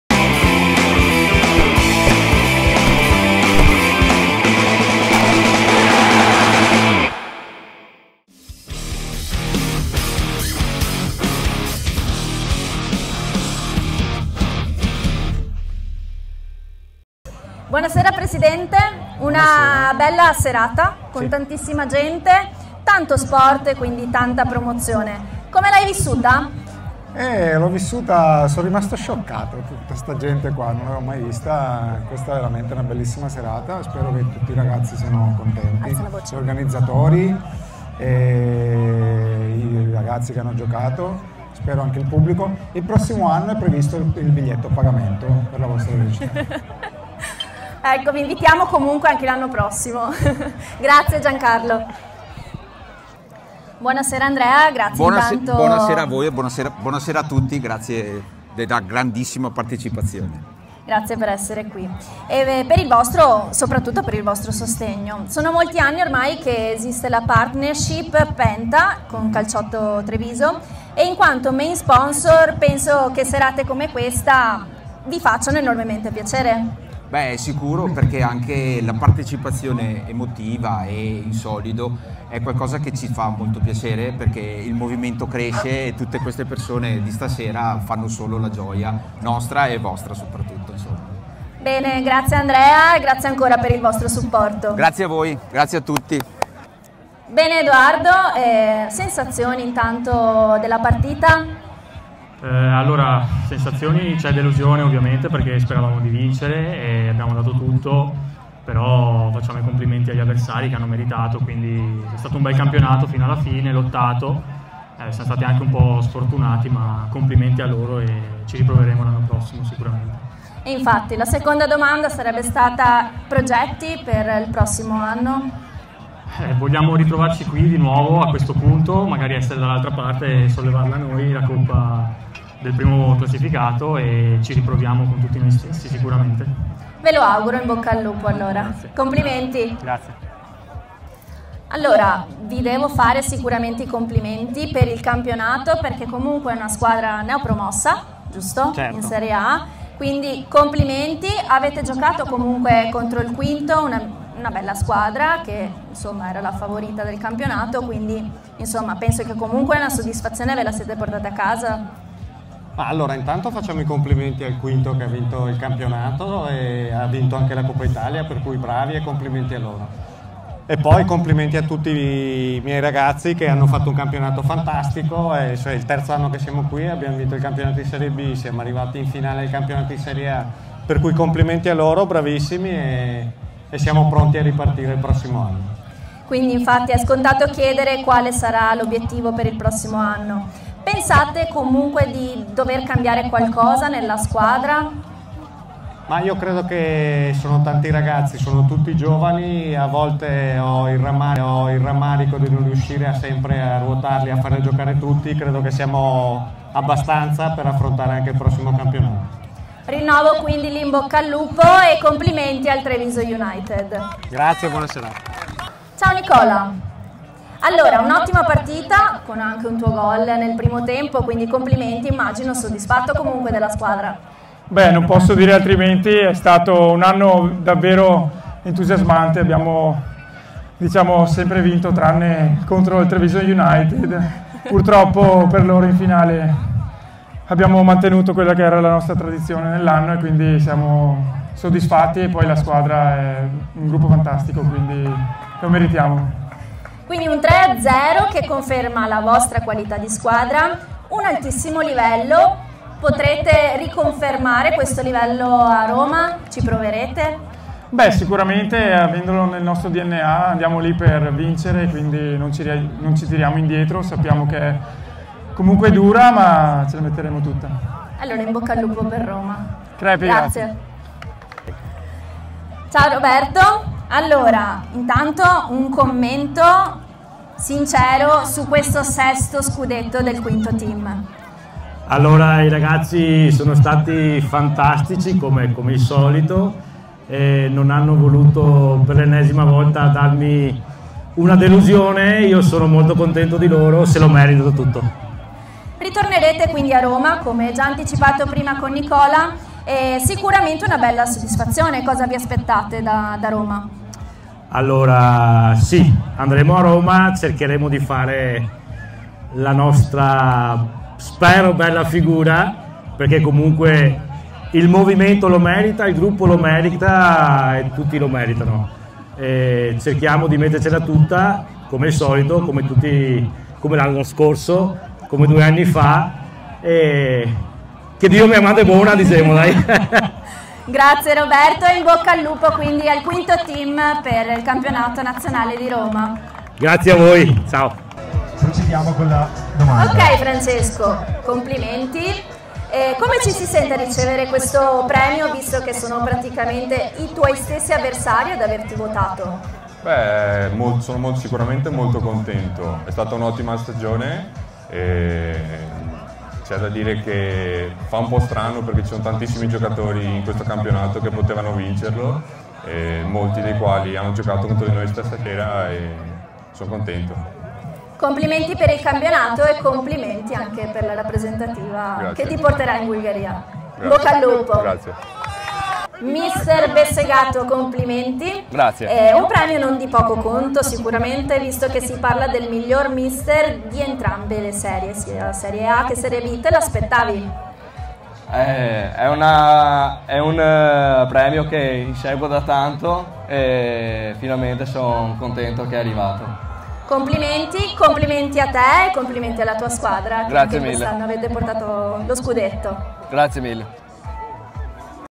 QUINTO TEAM A.S.D. vs TREVISO UNITED 3-0 – INTERVIEWS
BIG MATCH

Serie A – Finale – INTERVIEWS

QUINTO TEAM A.S.D. vs TREVISO UNITED 3-0 del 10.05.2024

Interviste a Giancarlo Mallia (Presidente CSEN), Andrea Gasparin (PENTA S.R.L.), Edoardo Guerra (TREVISO UNITED), al Mister Francesco Sama (TREVISO UNITED), Nicola Franchetto (QUINTO TEAM A.S.D.), al Presidente Roberto Zanella (QUINTO TEAM A.S.D.), Francesco Semenzato (TREVISO UNITED) e al Mister Denis Bessegato (QUINTO TEAM A.S.D.)

📍 CALCIOTTO TREVISO: l'unico movimento ufficiale di calcio a 8 in Veneto!